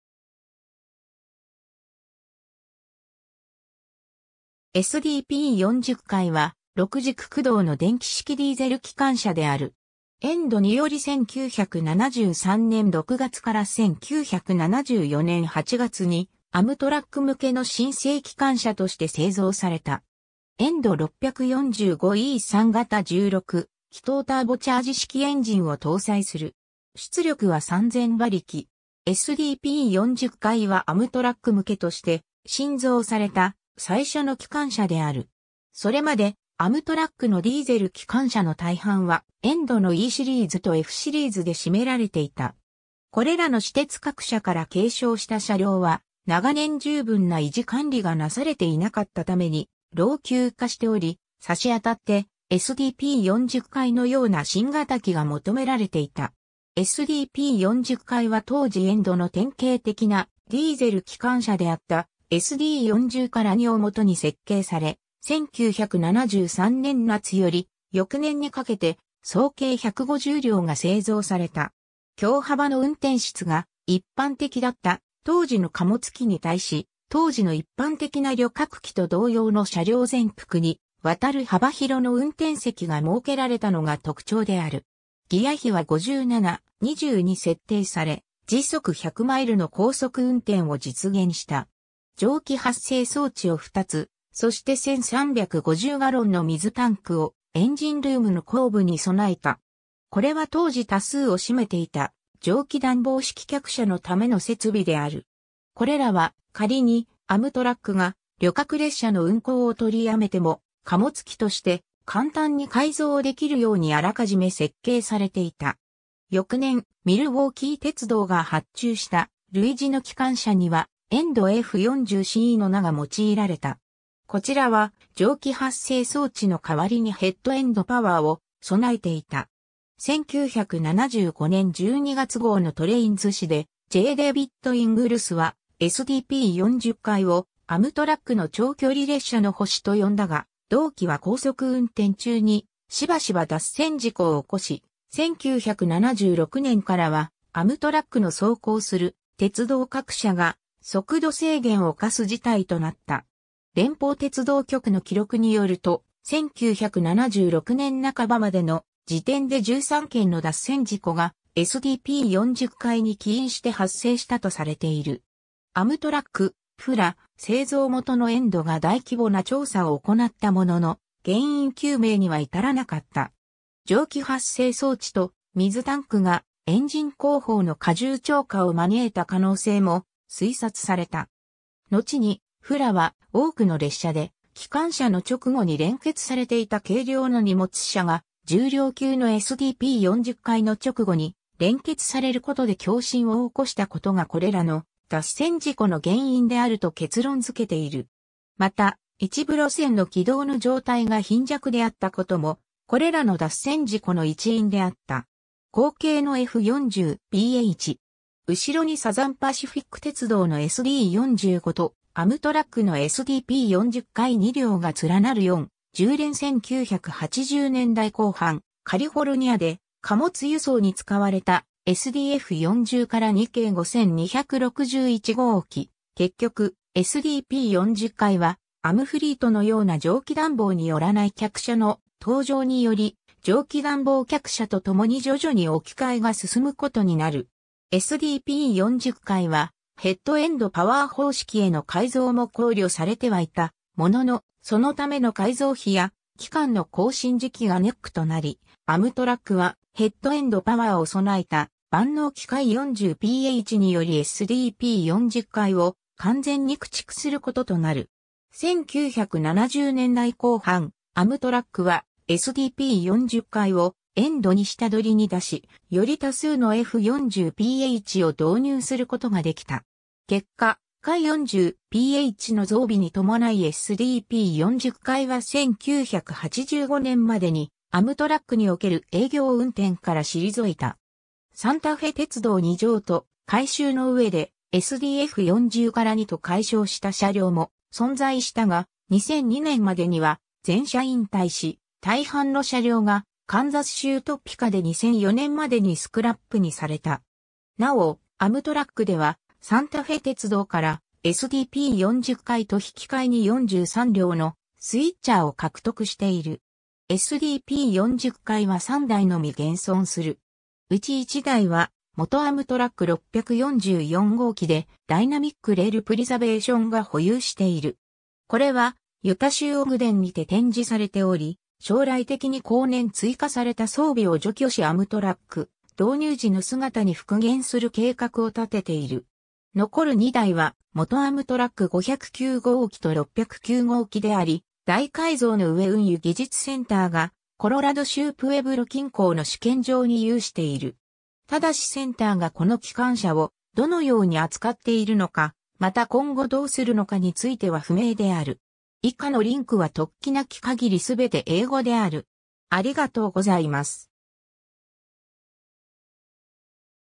EMD SDP40F形ディーゼル機関車
エレクトロ・モーティブ・ディーゼル製のディーゼル機関車
SDP40Fは、6軸駆動の電気式ディーゼル機関車である。
EMDにより1973年6月から1974年8月に、アムトラック向けの新製機関車として製造された。
EMD 645E3型16気筒ターボチャージ式エンジンを搭載する。
出力は3000馬力。
 SDP40Fはアムトラック向けとして新造された最初の機関車である。
 それまで、アムトラックのディーゼル機関車の大半はEMDのEシリーズとFシリーズで占められていた。
これらの私鉄各社から継承した車輌は、長年十分な維持管理がなされていなかったために老朽化しており、さしあたってSDP40Fのような新型機が求められていた。
 SDP40Fは当時EMDの典型的なディーゼル機関車であったSD40-2をもとに設計され、1973年夏より翌年にかけて総計150両が製造された。
狭幅の運転室が一般的だった当時の貨物機に対し、当時の一般的な旅客機と同様の車輌全幅にわたる幅広の運転席が設けられたのが特徴である。
ギヤ比は57:20に設定され時速100マイル (160 km/h)の高速運転を実現した。
 蒸気発生装置を2つ、そして 1350ガロン(5100リットル)の水タンクをエンジンルームの後部に備えた。
これは当時多数を占めていた蒸気暖房式客車のための設備である（この他に、車体台枠下部に2150ガロン(8100リットル)分の水タンクも有した）。
これらは、仮にアムトラックが旅客列車の運行を取りやめても、貨物機として簡単に改造できるようにあらかじめ設計されていた。
 翌年、ミルウォーキー鉄道が発注した類似の機関車にはEMD F40Cの名が用いられた。
こちらは蒸気発生装置のかわりにヘッド・エンド・パワーを備えていた。
 1975年12月号のTrains誌でJ.デイヴィッド・イングルスはSDP40Fを「アムトラックの長距離列車の星」("stars of Amtrak's long-distance trains")と呼んだが、同機は高速運転中にしばしば脱線事故を起こし、1976年からはアムトラックの走行する鉄道各社が速度制限を課す事態となった。
連邦鉄道局(FRA)の記録によると、1976年半ばまでの時点で13件の脱線事故がSDP40Fに起因して発生したとされている。
アムトラック、FRA、製造元のEMDが大規模な調査を行ったものの、原因究明には至らなかった。
蒸気発生装置と水タンクがエンジン後方の荷重超過を招いた可能性も推察された。
のちにFRAは、多くの列車で機関車の直後に連結されていた軽量の荷物車が、重量級のSDP40Fの直後に連結されることで共振を起こしたことがこれらの脱線事故の原因であると結論づけている。
また、一部路線の軌道の状態が貧弱であったことも、これらの脱線事故の一因であった。
 後継のF40PH（先頭）。
後ろにサザン・パシフィック鉄道のSD45とアムトラックのSDP40F2両が連なる4重連 1980年代後半、カリフォルニアで貨物輸送に使われたSDF40-2形5261号機 結局、SDP40Fはアムフリートのような蒸気暖房に依らない客車の登場により、蒸気暖房客車とともに徐々に置き換えが進むことになる。
 SDP40Fはヘッド・エンド・パワー方式への改造も考慮されてはいたものの、そのための改造費や機関の更新時期がネックとなり、アムトラックはヘッド・エンド・パワーを備えた万能機F40PHによりSDP40Fを完全に駆逐することとなる。
 1970年代後半、アムトラックはSDP40FをEMDに下取りに出し、より多数のF40PHを導入することができた。
結果、F40PHの増備に伴いSDP40Fは1985年までにアムトラックにおける営業運転から退いた。
 サンタフェ鉄道に譲渡、改修のうえでSDF40-2と改称した車輌も存在したが、2002年までには全車引退し、大半の車輌がカンザス州トピカで2004年までにスクラップにされた。
なお、アムトラックではサンタフェ鉄道からSDP40Fと引き換えに43両のスイッチャー（入換機）を獲得している。
 SDP40Fは3台のみ現存する。
うち1台は元アムトラック644号機で、ダイナミック・レール・プリザベーション（外部リンク節参照）が保有している。
これはユタ州オグデンにて展示されており、将来的に後年追加された装備を除去しアムトラック導入時の...